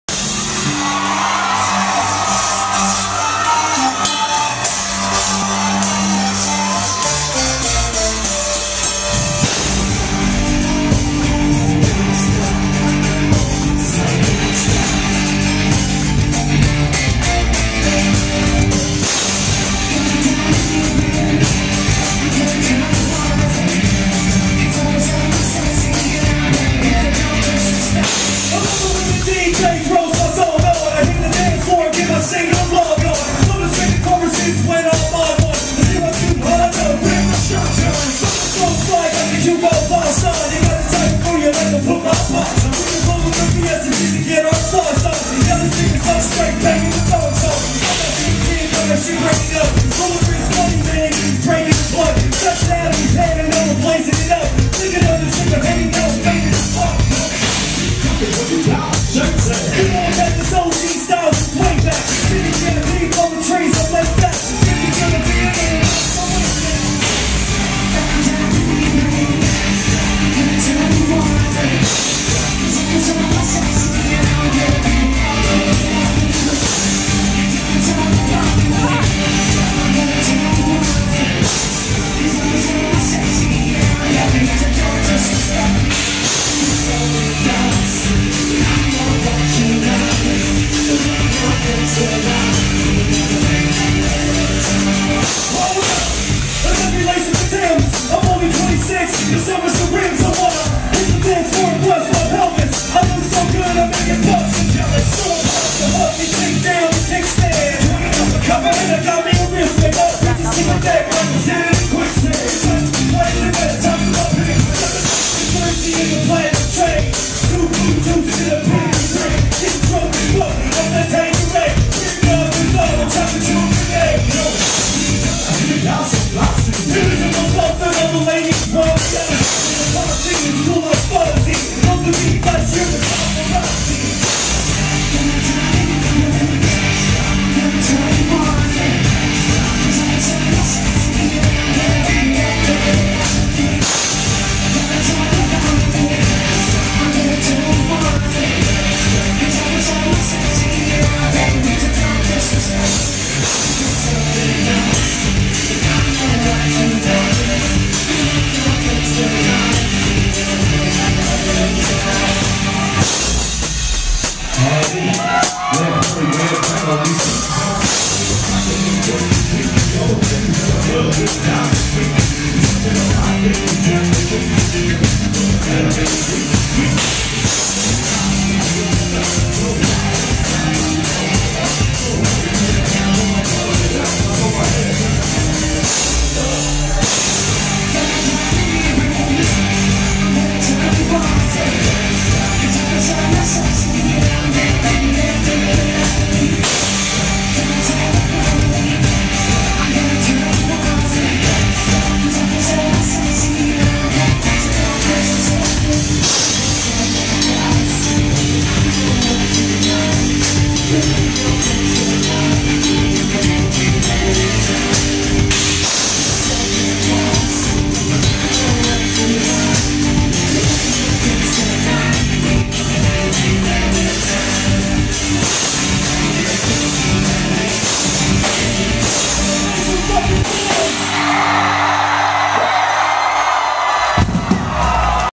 Hollywood Undead LIVE with BAL
Hollywood Undead put on a killer show for the House of Blues in Cleveland, Ohio on The Revolt Tour! Backstage Artist Lounge and CREAM (alcohol infused whipped cream 30 proof!) hooked up Hollywood Undead, along with 10 Years, Drive A and New Medicine with lots of fun swag backstage. On tour forever!!!